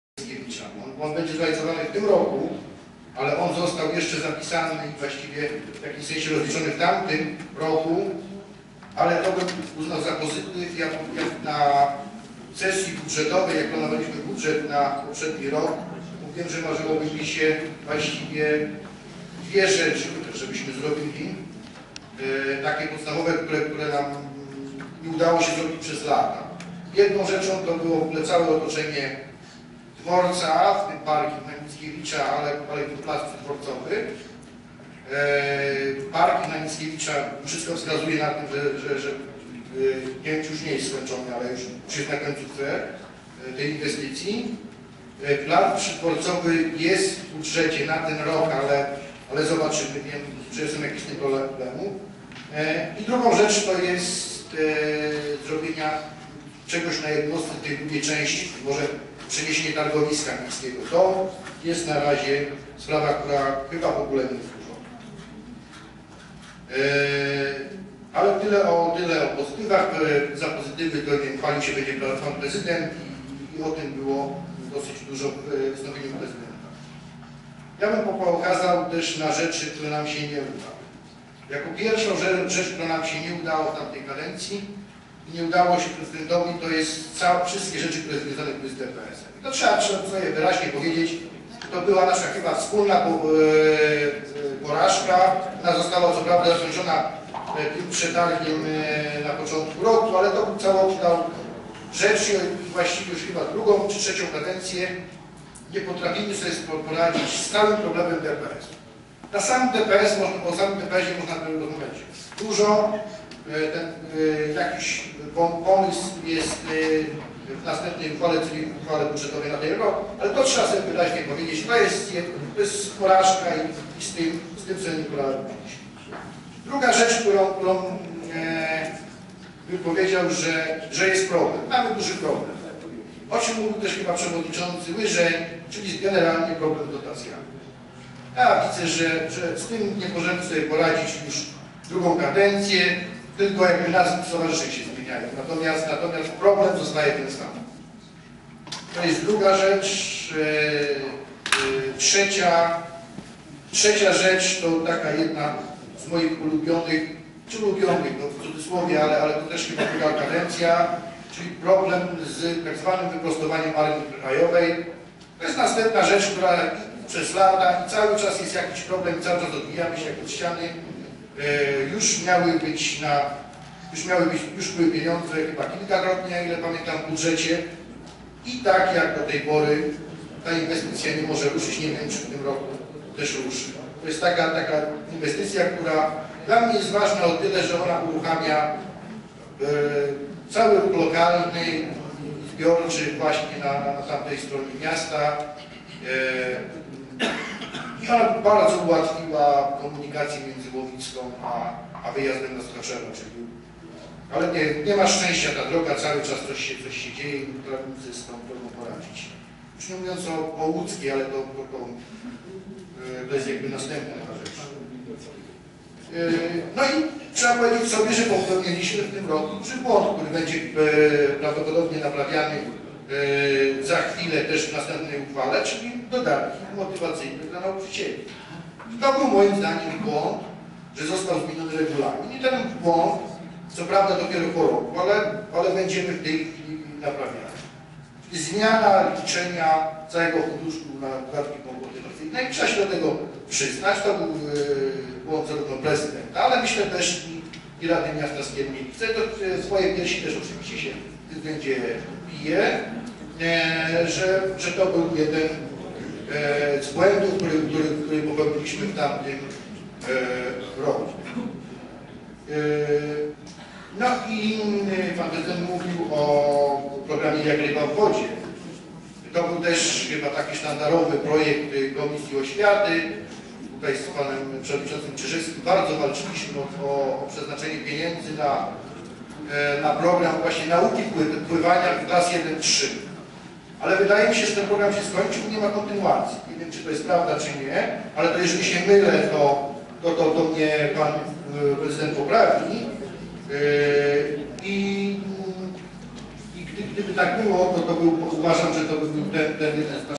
Absolutorium dla Prezydenta Leszka Trębskiego - Część 9 [26.04.2010]
Nagranie sesji Rady Miasta Skierniewice z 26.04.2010 na której odbyło się głosowanie za absolutorium dla urzędującego Prezydetna Leszka Trębskiego.